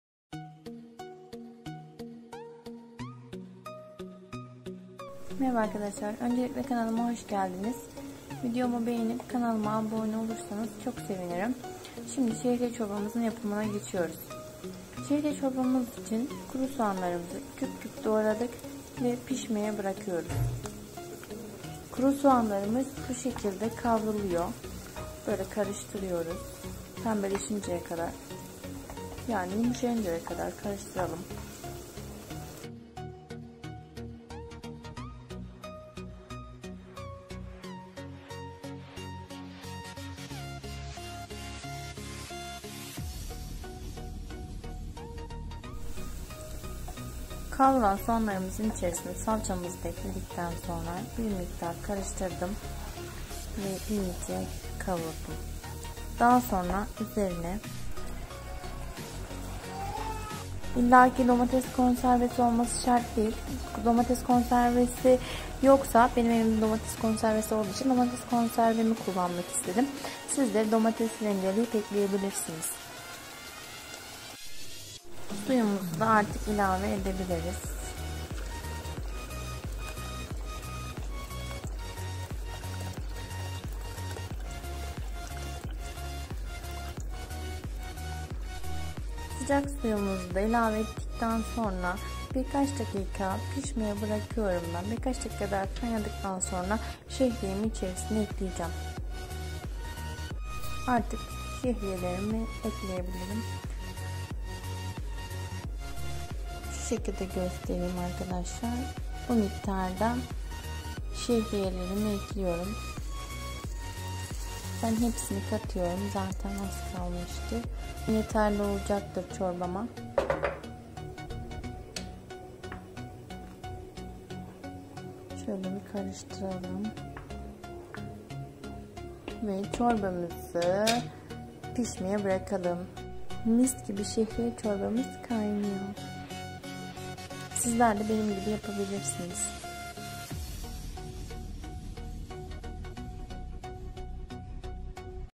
Miss gibi şehriye çorbası tarifi 💯🥣 2.tabağı da isteyeceksiniz 👌
şehriyeçorbası için gerekli malzemeler

1 adet soğan
1 kaşık salça
1 çay bardağı arpa şehriye
domates konservesi yada domates rendesi
Su
istediğiniz baharatları ekleyebilirsiniz